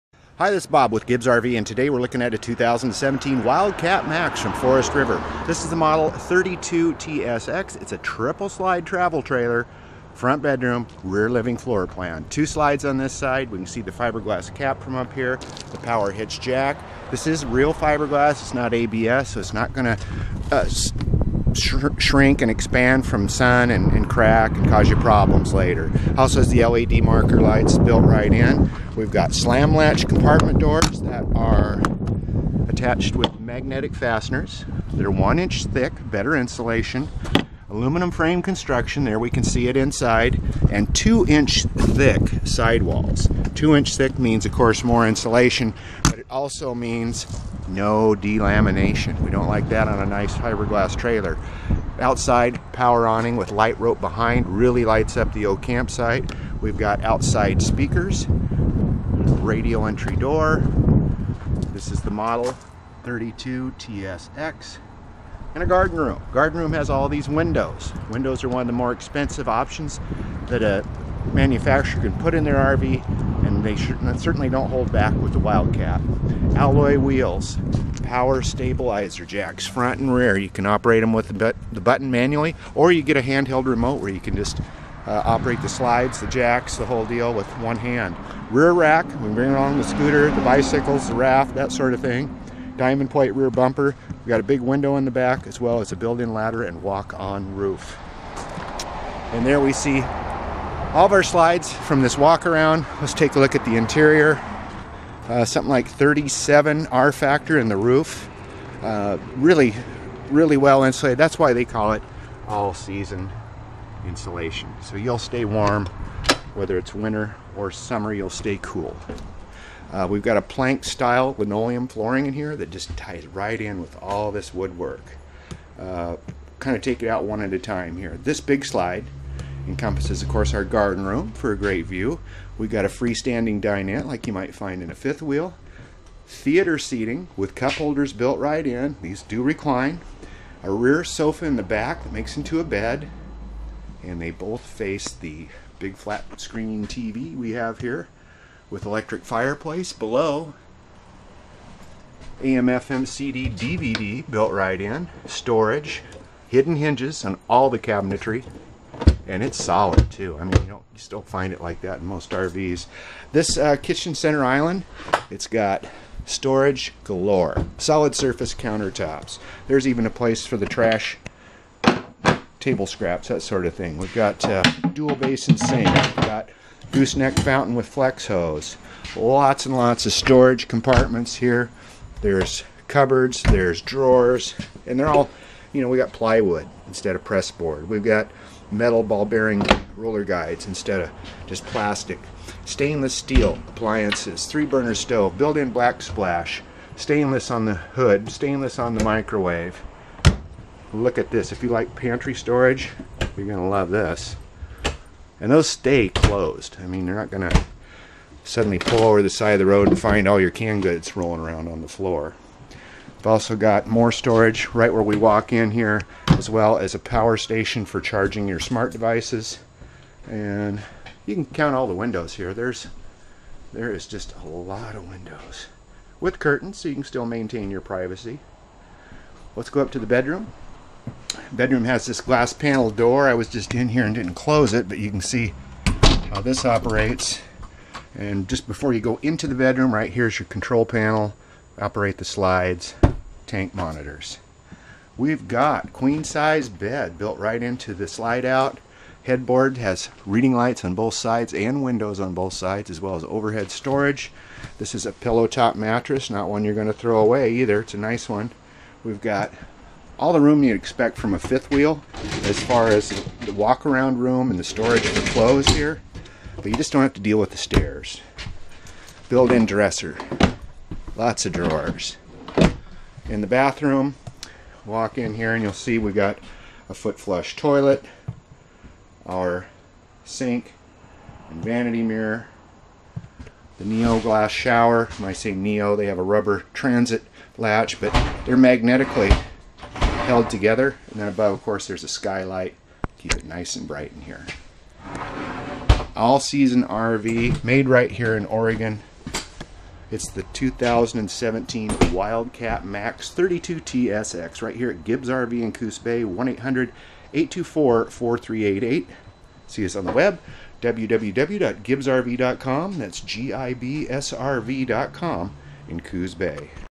2017 Wildcat 32TSX #017261
Triple slide travel trailer with rear living floor plan & lots of windows. Treat yourself to the look & feel of solid surface counter tops and elegant decor that surrounds you in this Wildcat 32TSX All Season RV!